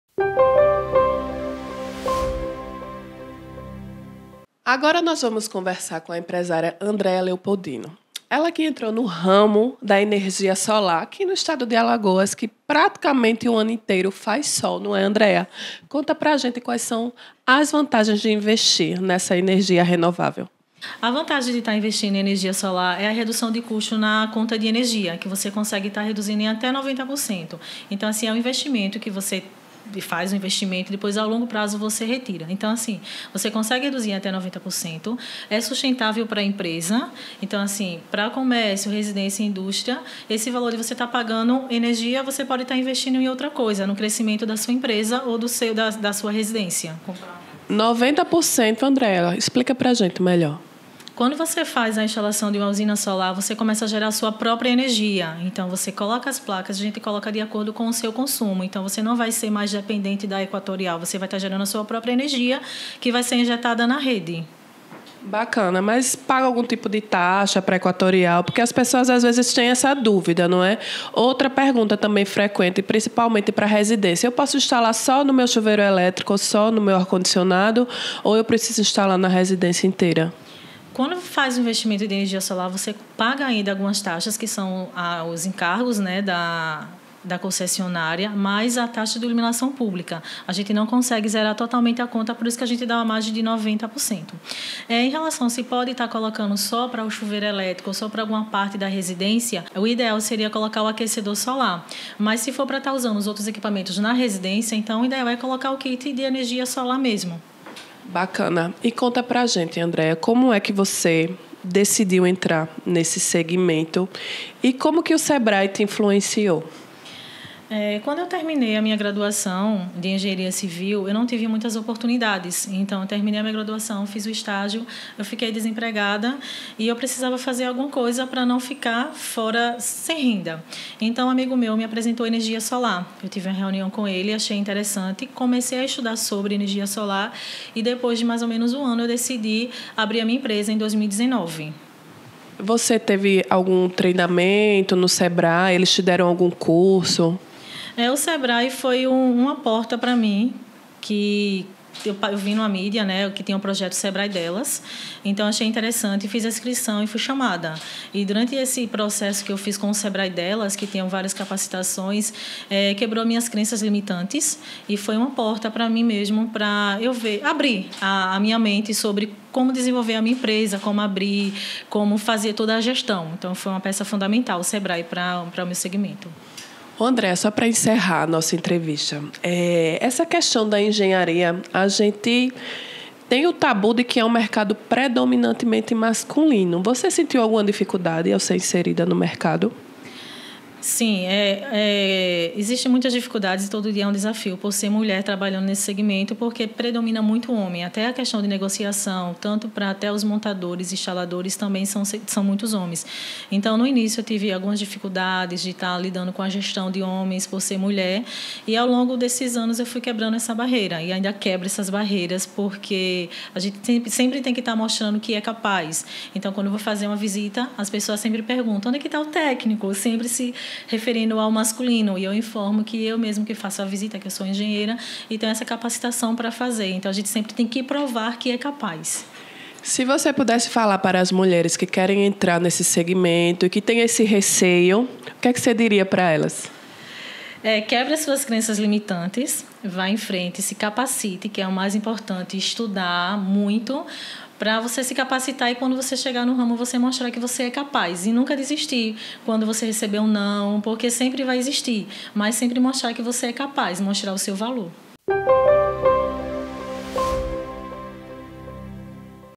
Tribuna Hoje - Entrevista com Andrea Leopoldino
Tribuna Hoje  - Entrevista com a engenheira civil Andrea Leopoldino